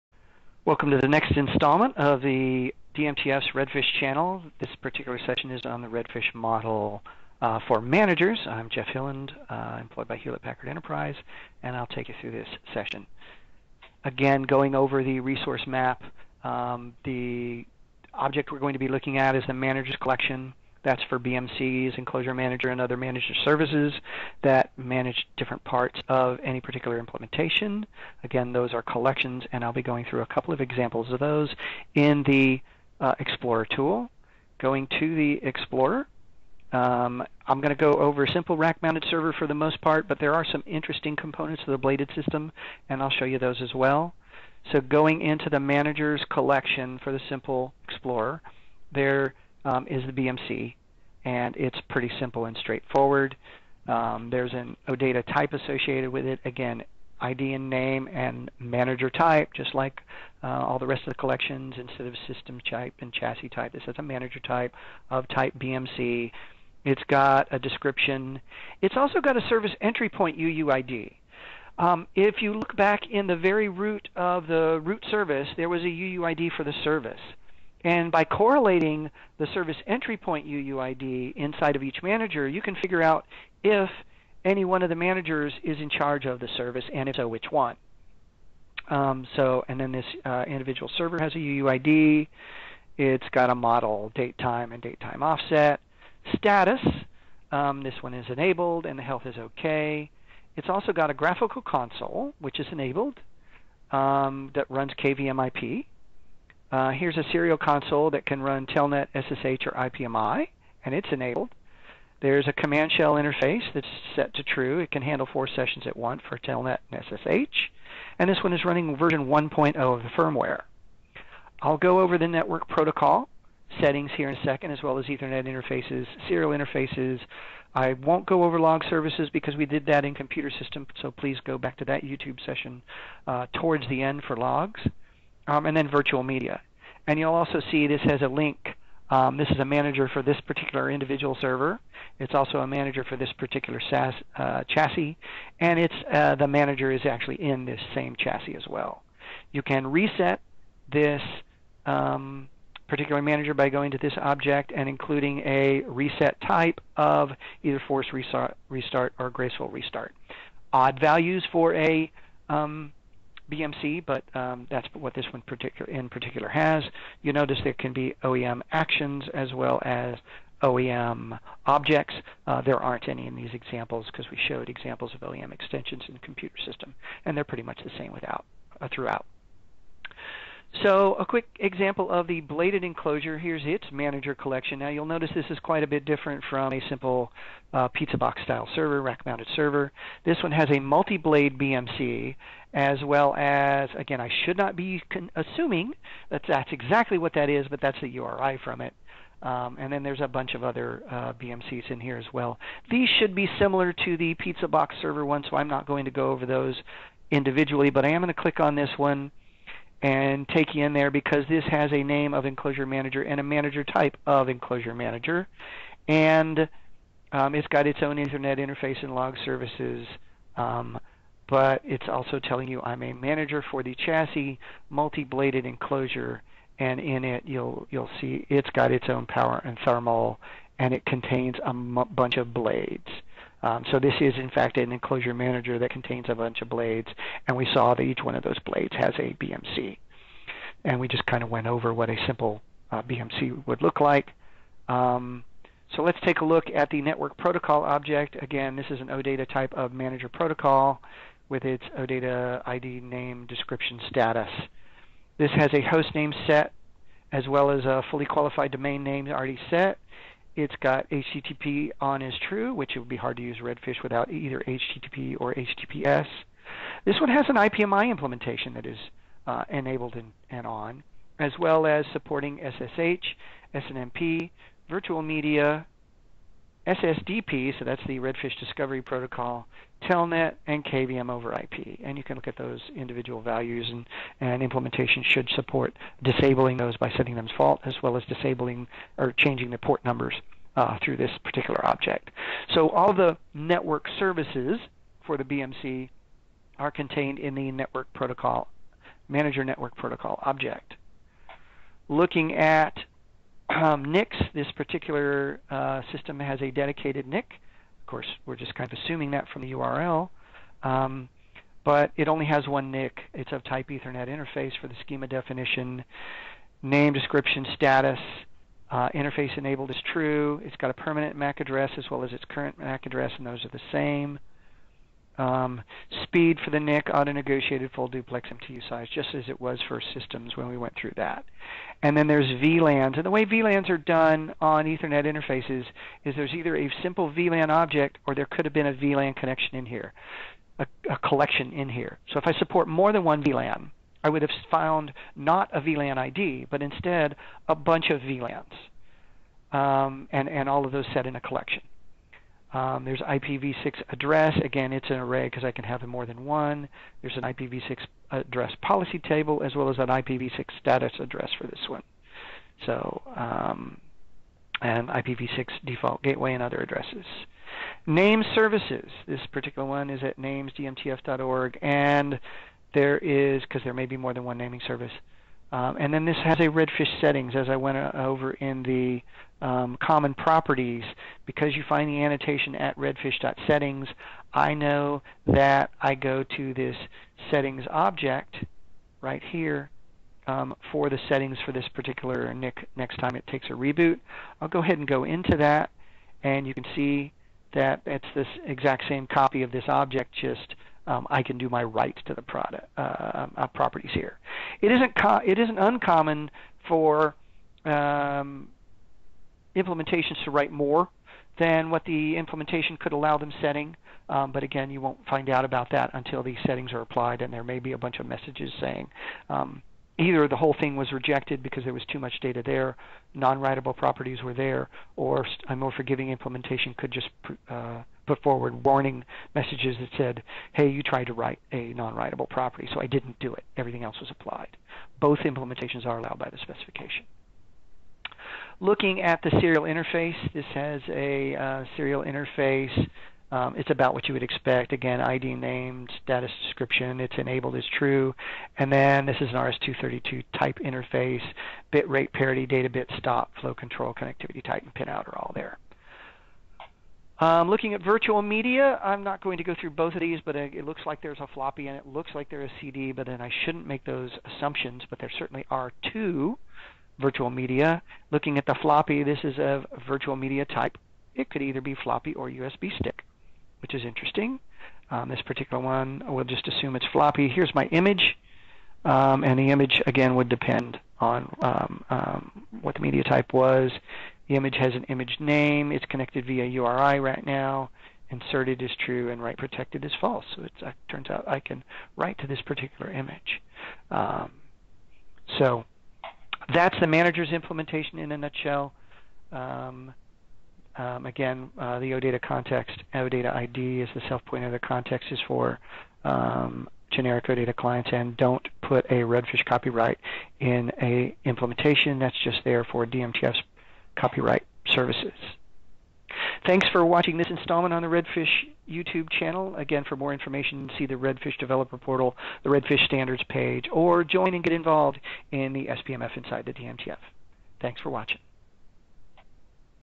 Redfish™ Model: Managers
Learn more about managers in DMTF’s Redfish, a standard that helps enable simple and secure management of modern scalable platform hardware. This video explains what a manager is, as well as the services and devices commonly associated with a manager.

Please Note: The DMTF group formerly known as the Scalable Platforms Management Forum (SPMF) has been renamed the Redfish Forum.